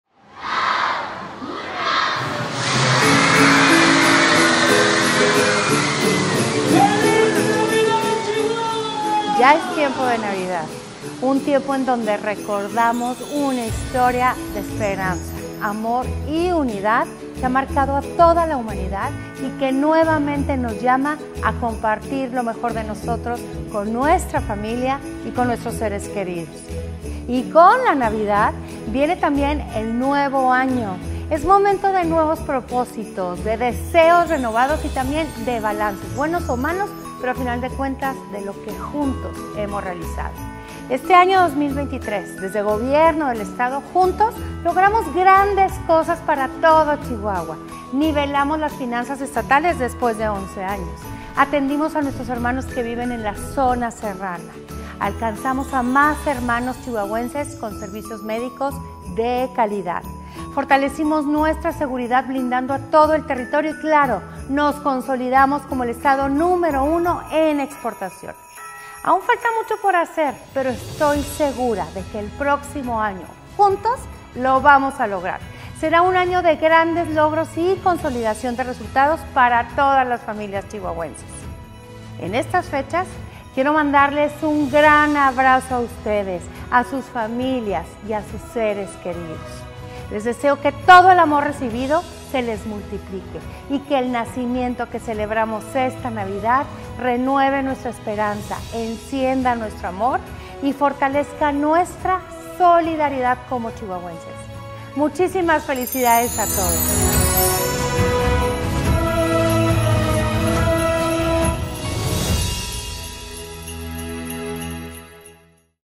Feliz navidad y prospero año nuevo.
Estas fechas llegan con nuevos propósitos, deseos renovados y recuerdos de lo que hemos logrado.

En el Gobierno del Estado, este 2023, juntos hemos alcanzado grandes resultados en salud, economía, seguridad y mucho más.

Falta mucho por hacer, pero lo lograremos juntos este 2024.